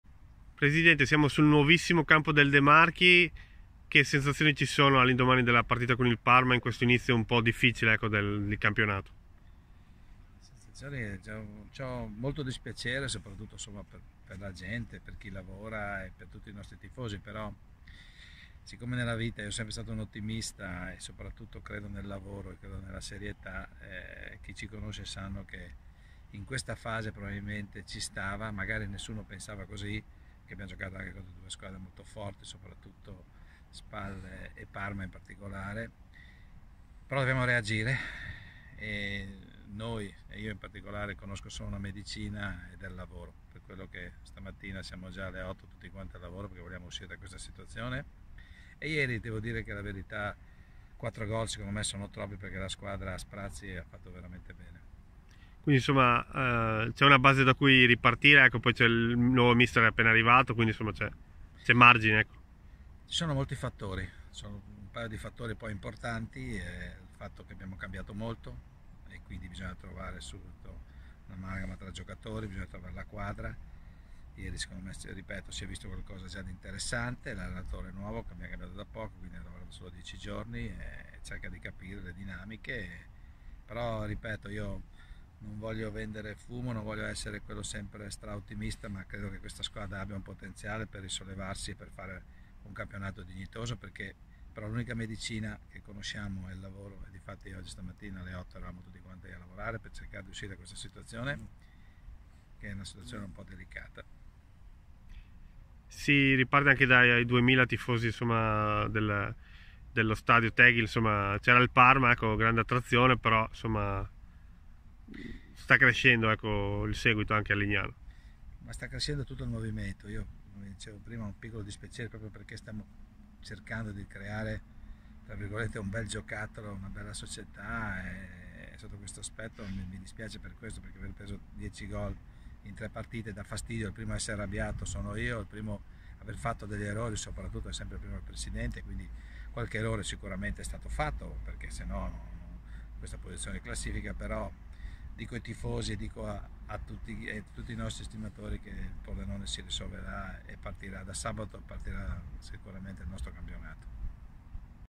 MESSAGGIO DEL PRESIDENTE LOVISA
"Sono molto dispiaciuto, in primis per i tifosi, ma usciremo presto da questa situazione": il messaggio del presidente Mauro Lovisa all'indomani della sconfitta con il Parma 💪🏻🖤💚